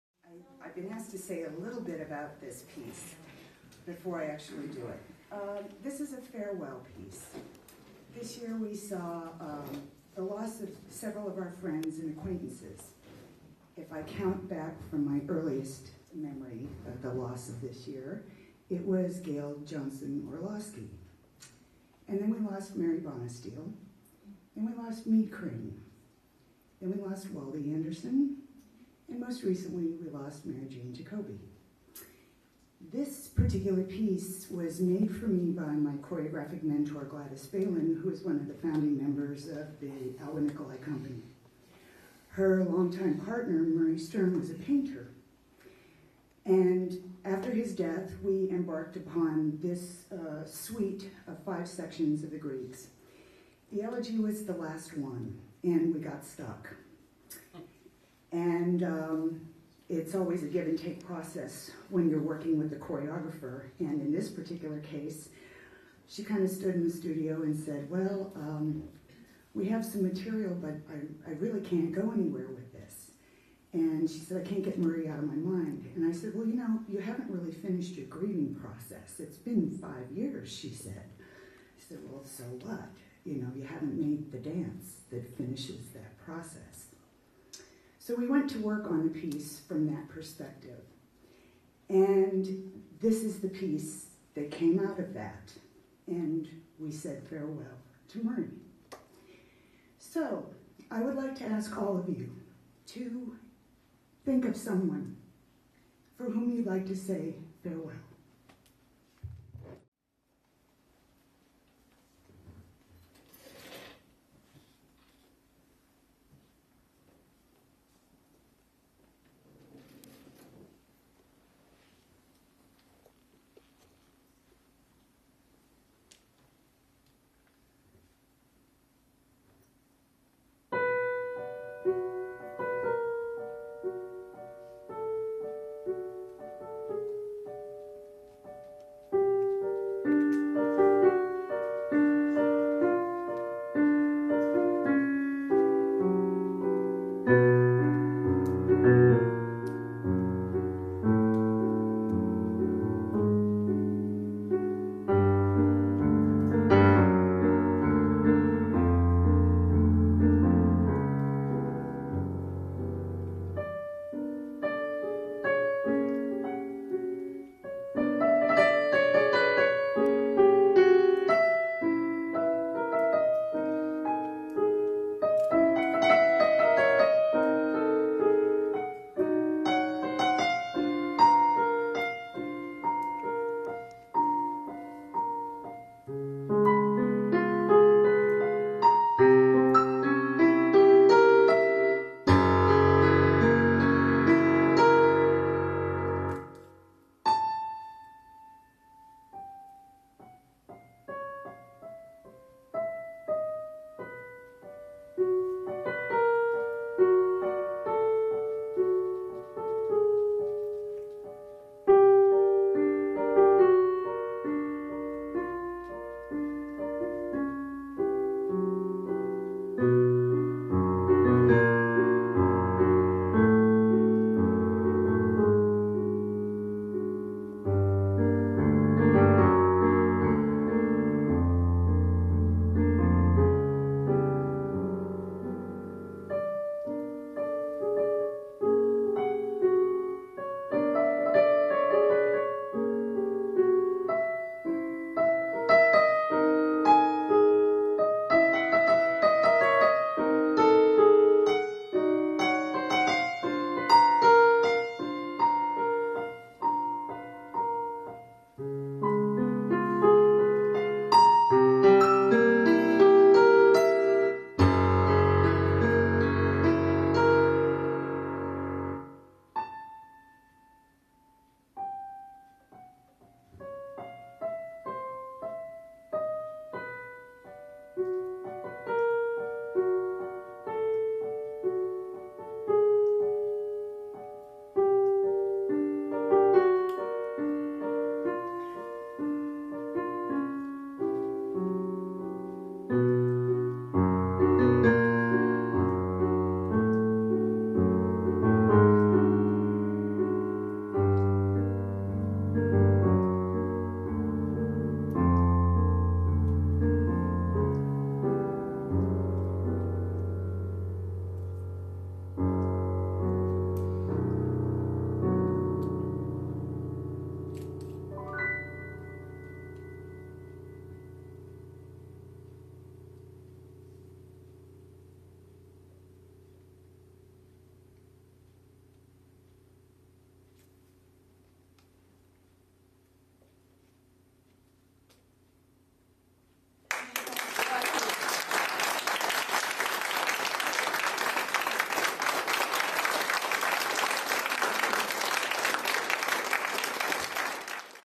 Farewell: A Tribute To IAA Deceased Alumni
Farewell: A tribute to Interlochen Arts Academy alumni who have passed away during the past year:
Gayle Johnson Orloski
Mary Bonisteel
Meade Crane
Waldi Anderson
Mary Jane Jacobi

Performers in this video:
Marina Gobins Walchli - dance
James Carter Cathcart - Yamaha electronic piano

Interlochen Arts Academy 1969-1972 or so
August 2011 "Unofficial" Reunion
Golden Fellowship Hall
Interlochen, Michigan